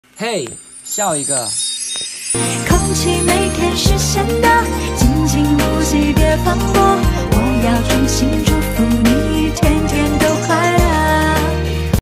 3 horse angle loading trailer horse float
The weather is getting colder , come and customize a comfortable trailer for your horse ~